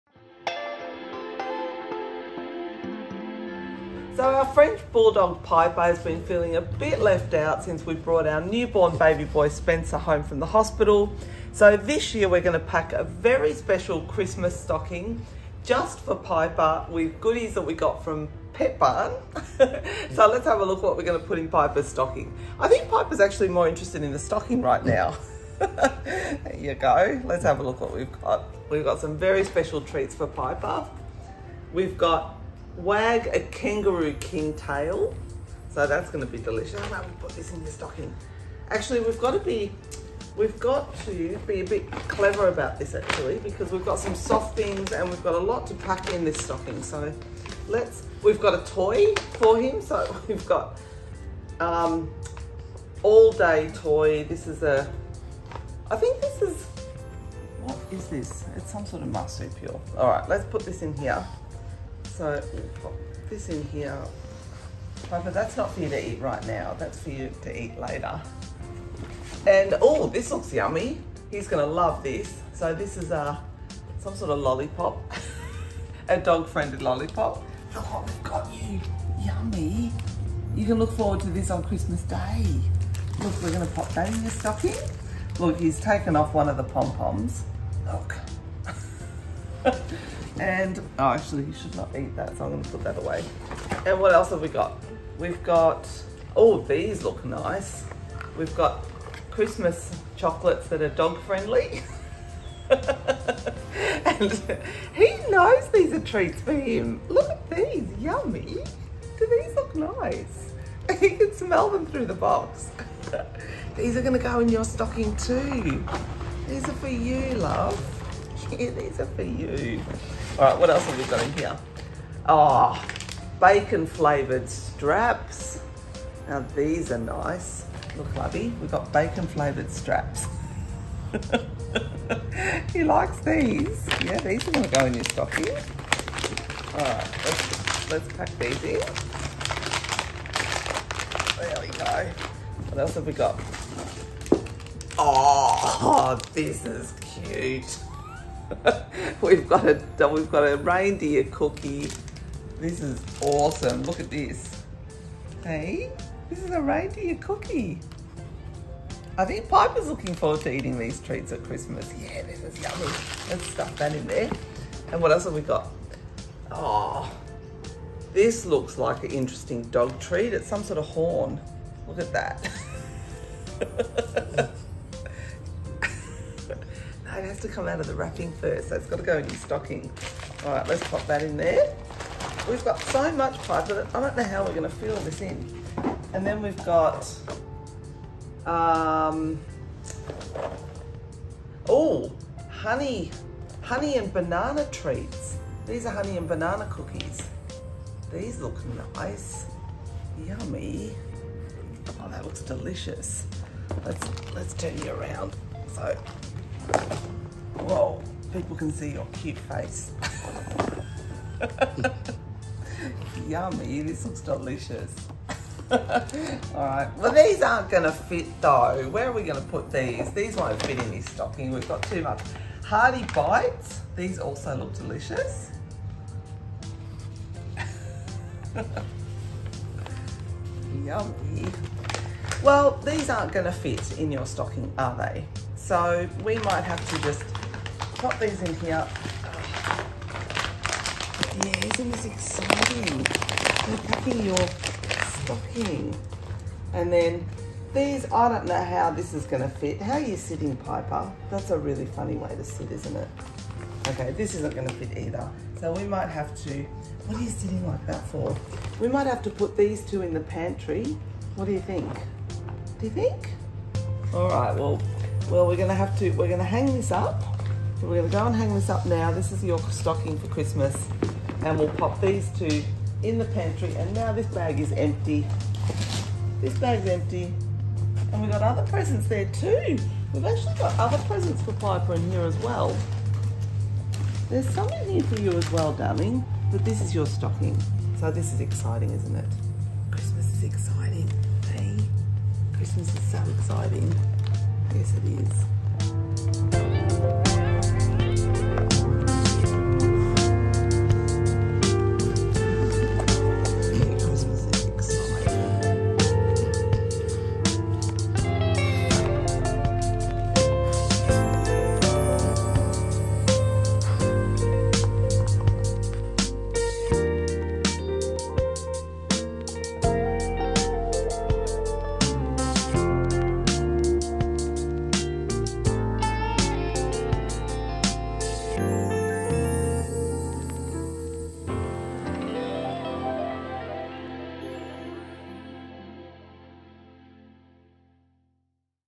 FILLING OUR FRENCH BULLDOG PIPER'S CHRISTMAS STOCKING//WHAT TO PUT IN YOUR DOG'S CHRISTMAS STOCKING
Our beloved French bulldog Piper has been feeling left out since we brought our new-born baby boy Spencer home from the hospital. So, to remind him how special we think he is, we have stuffed a Christmas stocking this year that's just for him :)